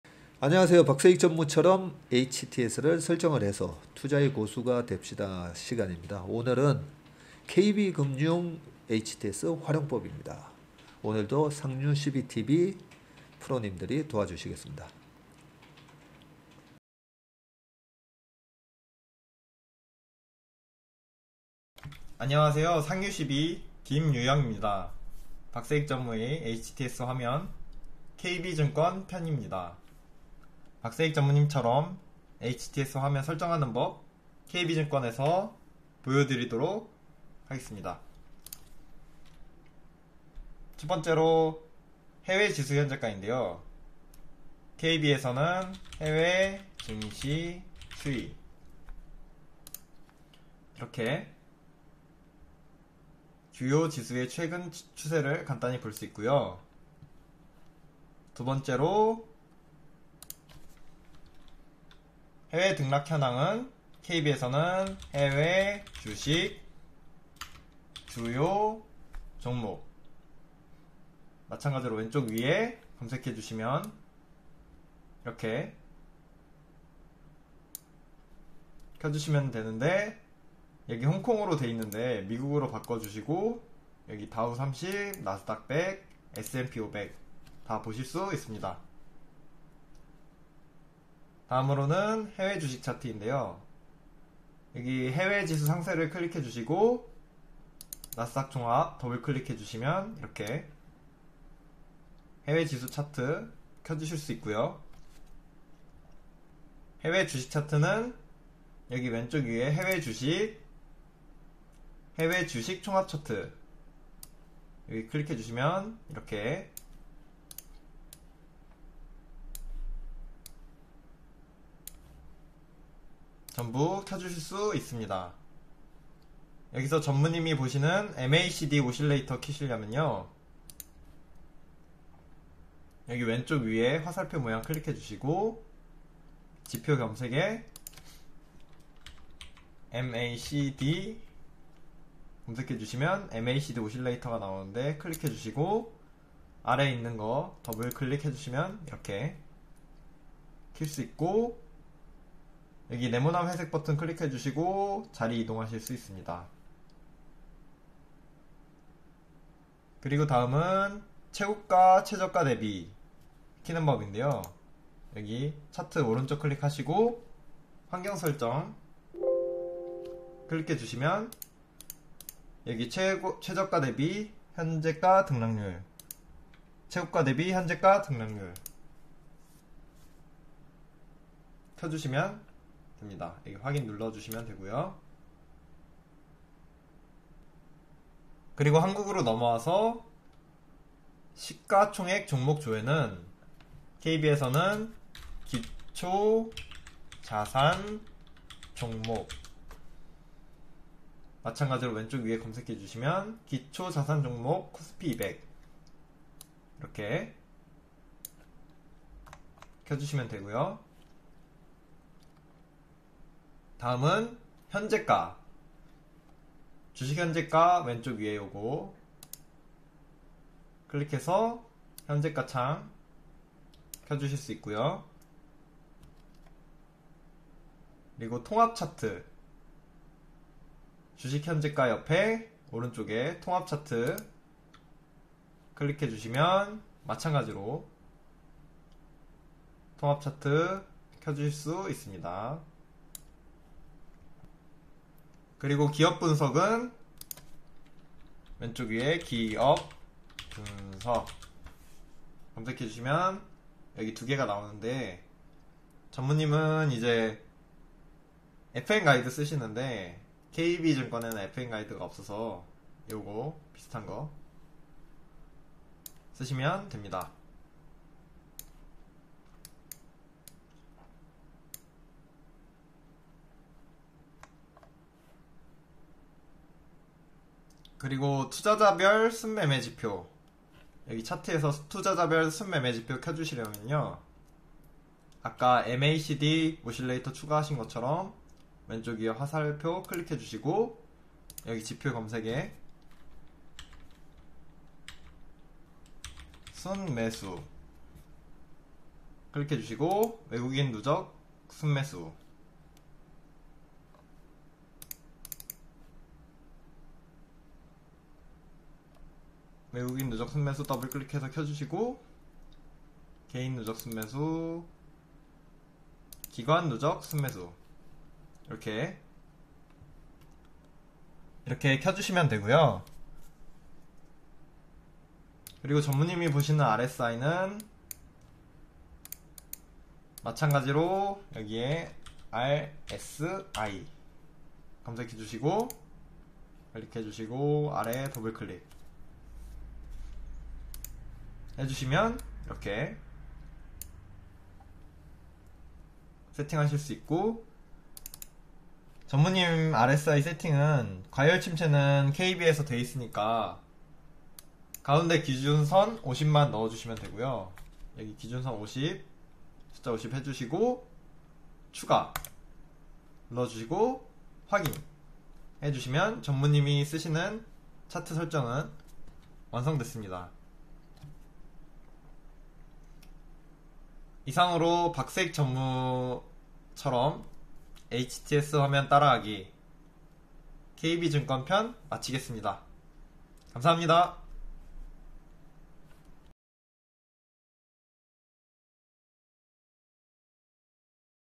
KB증권 HTS 박세익 전무처럼 설정하기(KB증권hts 세팅)
박세익 전무가 보는  HTS 설정 화면
초보도 따라 할 수 있게 차트 지표 설명까지

  ● 시가총액 상위 종목
  ● 차트설정
  ● RSI 박세익 전무 설정
  ● KB증권hts 세팅

위 항목에 관해 설명을 드립니다. 박세익 전무님이 HTS 화면에 나오는 정보들을 KB증권에서 비슷하게 설정하실 수 있도록 했습니다.

설명: 김유영